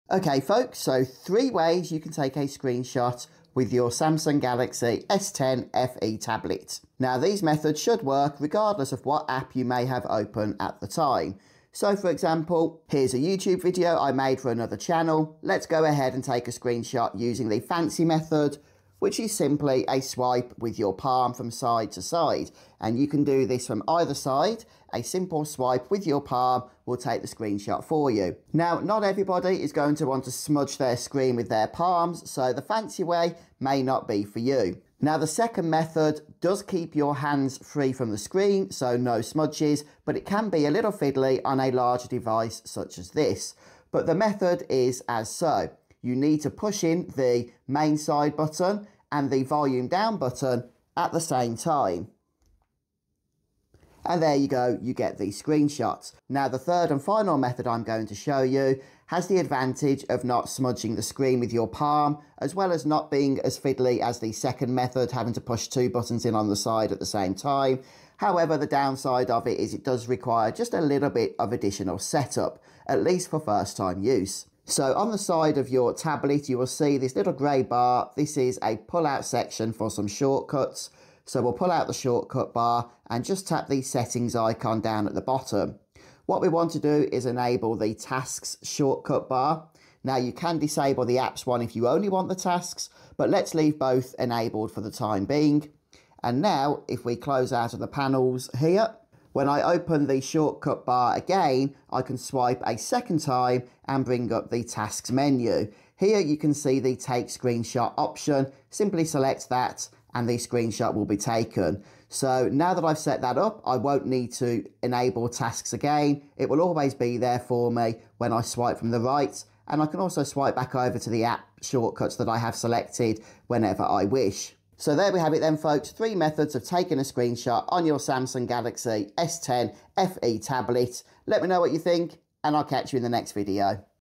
How to Take Screenshots on Samsung Galaxy Tab S10 FE (3 Easy Methods)
In this quick tutorial, I show you three simple ways to take screenshots on the Samsung Galaxy Tab S10 FE.

🔥 Samsung Galaxy S10 FE ➡️
(Commissions Earned)

 - Simon.